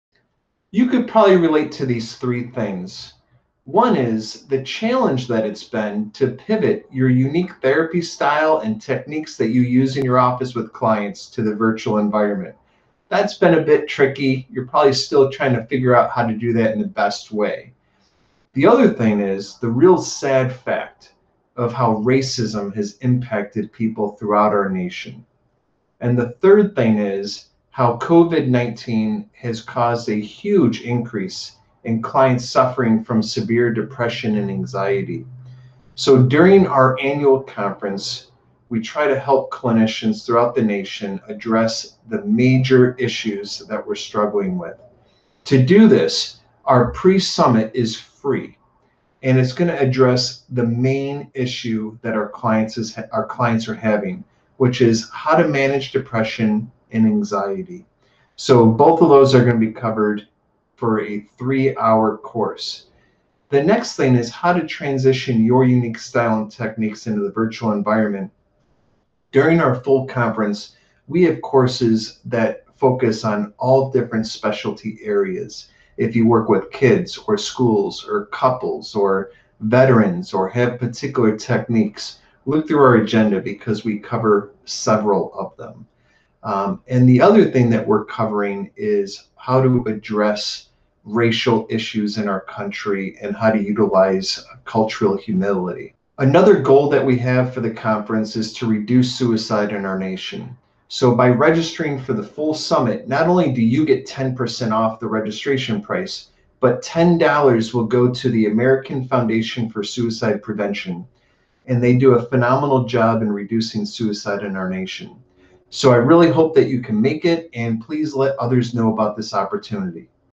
2021 TeleMental Health Summit
This video presented the 2021 Telemental Health yearly summit.